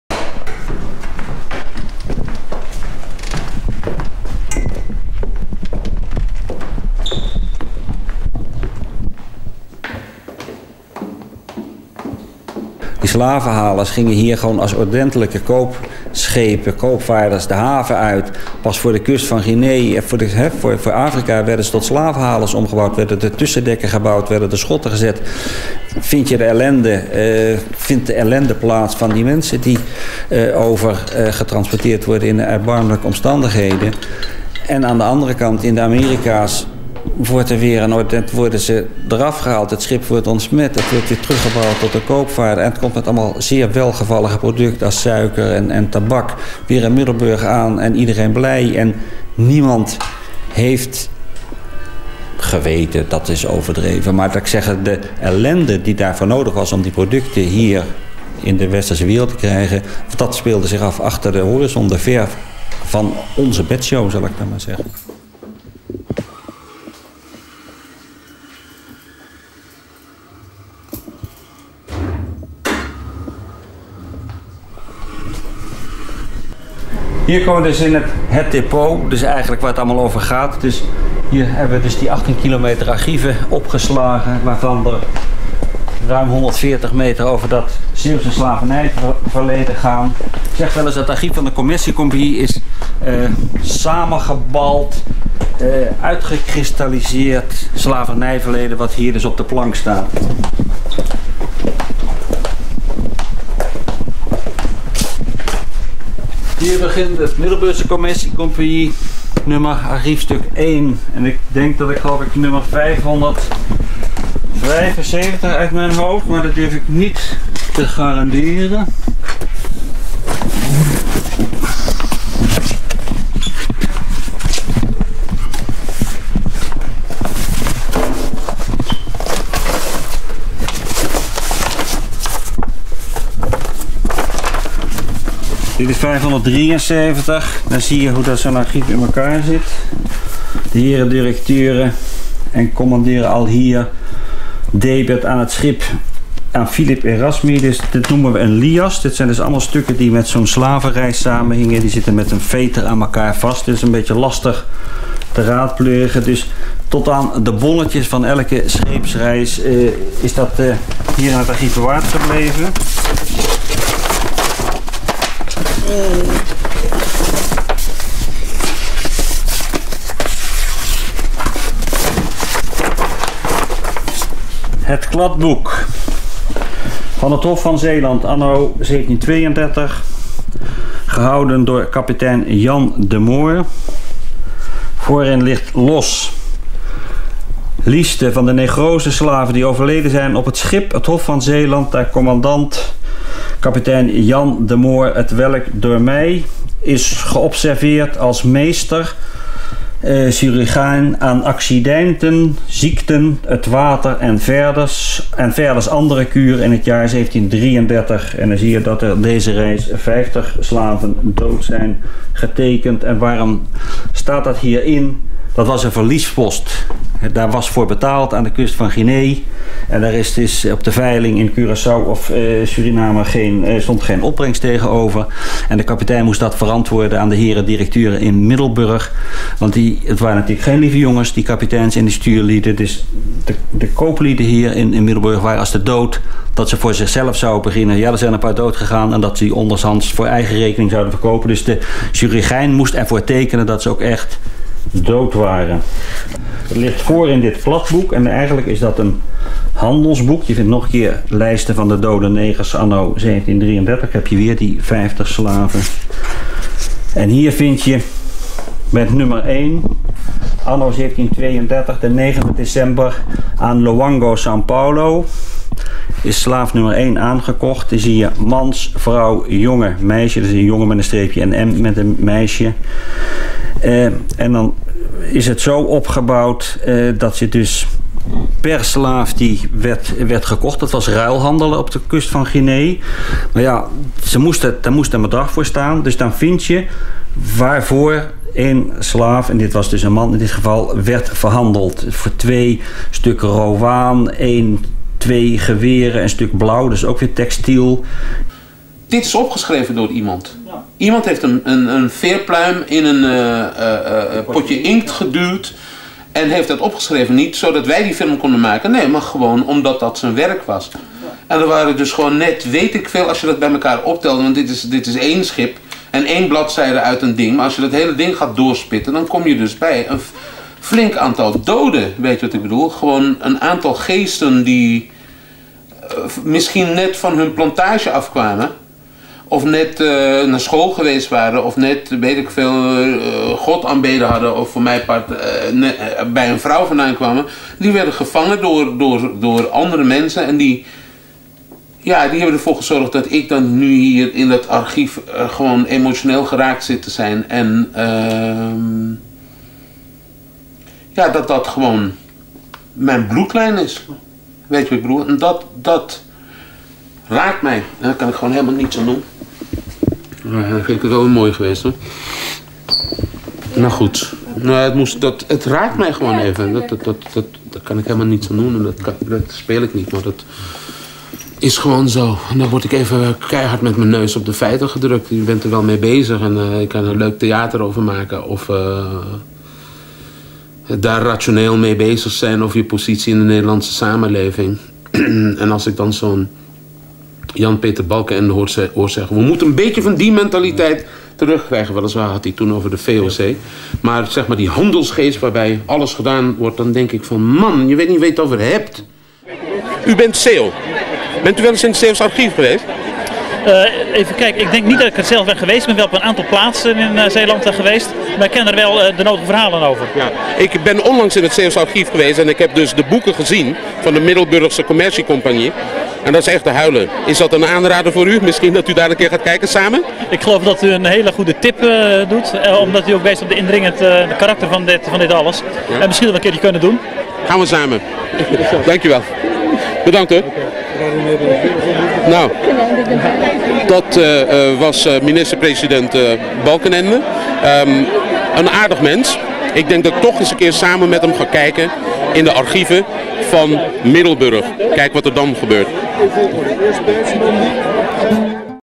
Slavernij Document Nu - Roelof Koops
Slavernij Document Nu - 2003In 2003 heb ik een documentaire gemaakt over slavernij. Daar in ben ik met de toenmalige directeur van het Zeeuws archief, Roelof Koops, de kelder in gedoken vaan de Middelburgse Commercie Compagnie. Een deel van de administratie van dat slavernijverleden ligt daar in een constante temperatuur van 19,7 graden Celcius in een luchtdicht gedeelte opgeslagen. Laat al die zeikerds die met de grootste kant van hun bek roepen dat de rijkdom van Nederland niet van slavernij komt ff hiernaar kijken. Godverdomme!